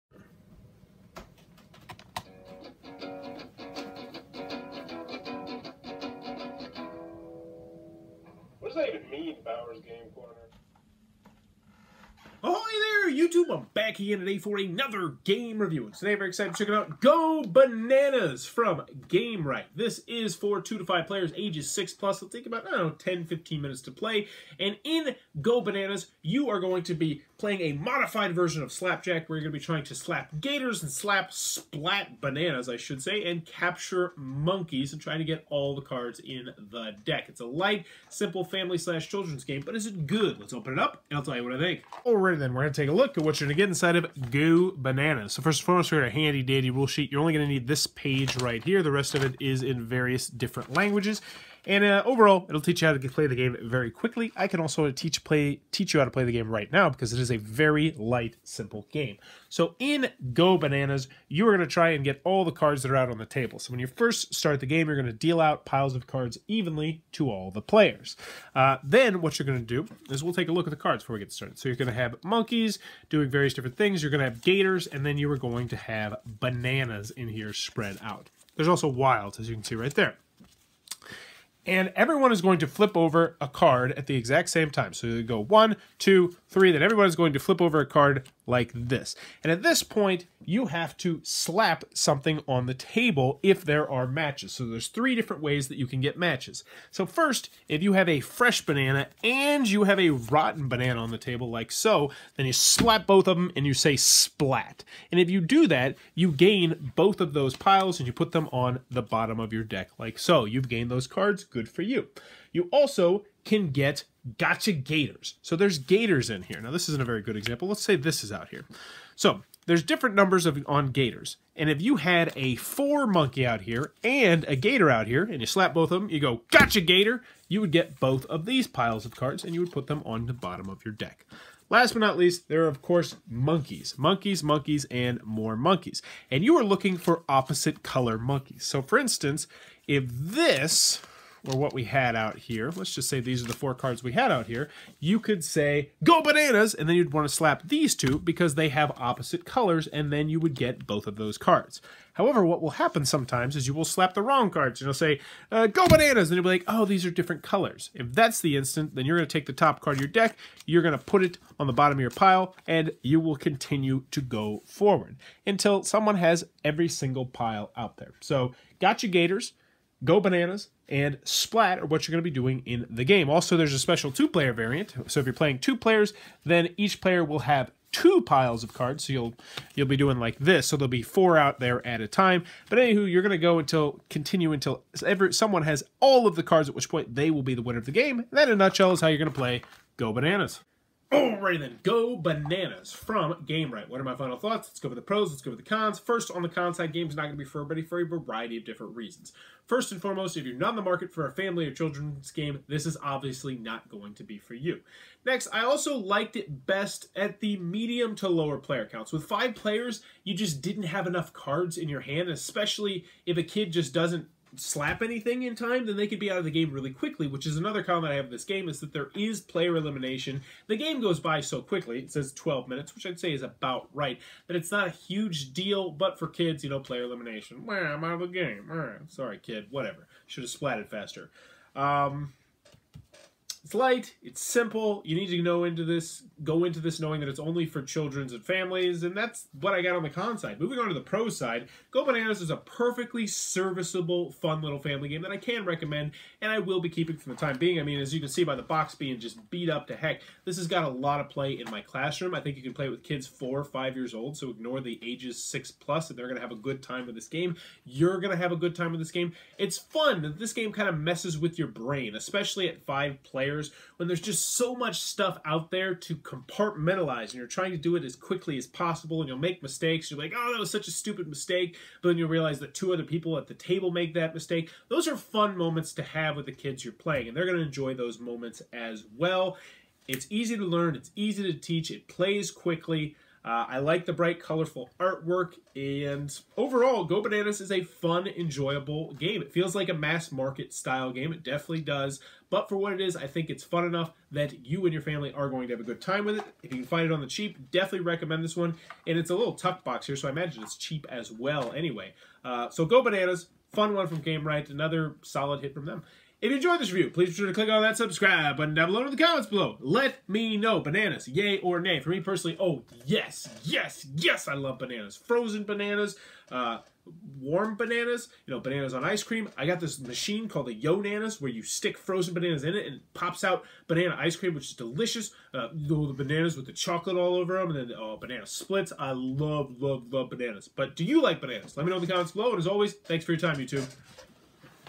Bower's Game Corner #830: Go Bananas! Review
From The Publisher:  The deck of 55 cards includes:
20 monkey cards with wild monkeys (red cards numbered 1 to 4); 20 monkey cards with mild monkeys (blue cards numbered 1 to 4); 8 Gotcha Gator cards; 6 banana cards (3 rotten and 3 fresh) and one Wild Gotcha Gator card.

The deck is dealt out to all players who make a face down pile in front of them. Players turn over the top card of their decks simultaneously and place the card in the center of the table. Players attempt to be the first to spot one of two types of matches: Two different colored cards with the same value on different players' piles OR two different colored cards of the same value on one player's pile. The first player to spot it slaps the pile and shouts "Go Bananas!" That player then takes the pile(s).

Additionally, players must watch out for matching Gotcha Gator cards (same number as card previously played in one player's pile) and rotten banana/fresh banana matches as well - each having its own shout as well.

The first player to collect all the cards in the deck is the winner.